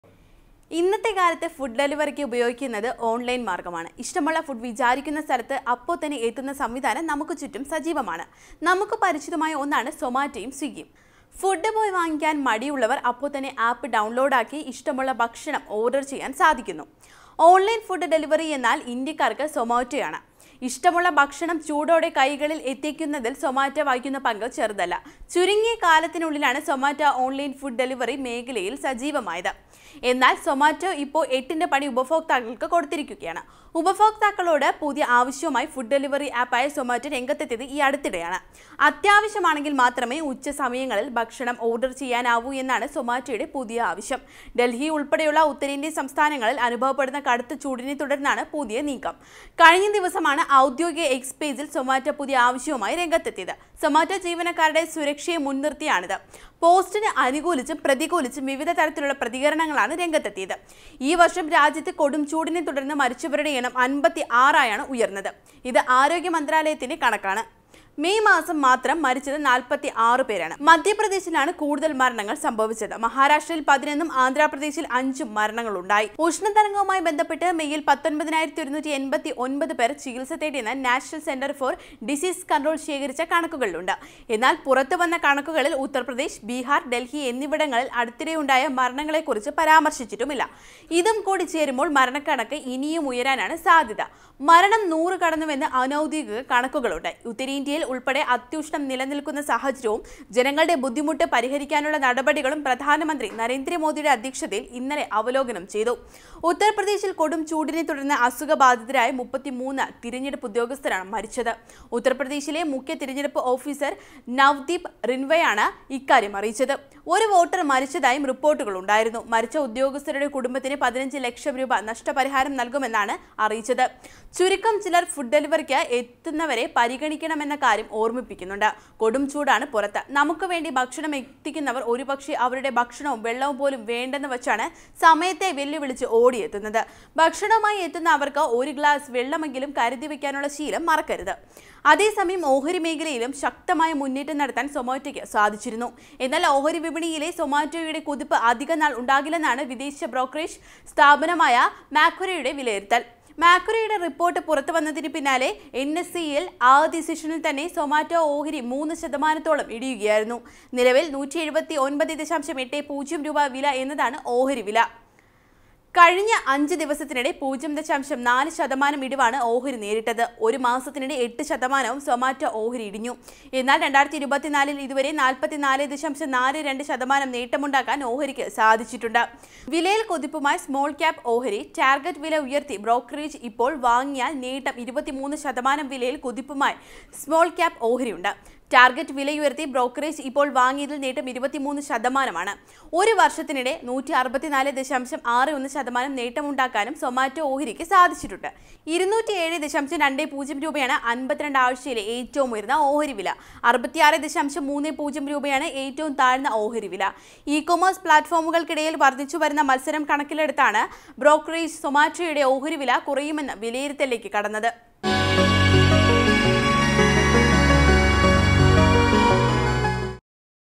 സൊമാറ്റോ ഇനി ഭക്ഷണം വിളമ്പില്ല, രാജ്യം നടുങ്ങി | Heatwave | Zomato | India
ഇന്നത്തെ കാലത്ത് ഫുഡ് ഡെലിവരിക്കും ഉപയോഗിക്കുന്നത് ഓണ്‍ലൈന്‍ മാര്‍ഗമാണ്. ഇഷ്ടമുള്ള ഫുഡ് വിചാരിക്കുന്ന സ്ഥലത്ത് അപ്പൊ തന്നെ എത്തുന്ന സംവിധാനം നമ്മുക്ക് ചുറ്റും സജീവമാണ്. നമുക്ക് പരിചിതമായ ഒന്നാണ് സോമാറ്റോ യും സ്വിഗിയും. ഫുഡ് പോയി വാങ്ങിക്കാന്‍ മടി ഉള്ളവര്‍ അപ്പൊ തന്നെ ആപ്പ് ഡൌണ്‍ലോഡ് ആക്കി ഇഷ്ടമുള്ള ഭക്ഷണം ഓര്‍ഡര്‍ ചെയ്യാന്‍ സാധിക്കും. ഓണ്‍ലൈന്‍ ഫുഡ് ഡെലിവറി എന്നാല്‍ ഇന്ത്യക്കാര്‍ക്ക് സൊമാറ്റോ ആണ്. ഇഷ്ടമുള്ള ഭക്ഷണം ചൂടോടെ കയ്യികളില്‍ എത്തിക്കുന്നതില്‍ സൊമാറ്റോ വഹിക്കുന്ന പങ്ക് ചെറുതല്ല. ചുരുങ്ങിയ കാലത്തിന് ഉള്ളിലാണ് സൊമാറ്റോ ഓണ്‍ലൈന്‍ ഫുഡ് ഡെലിവറി മേഖലയിലെ രാജാവായത്.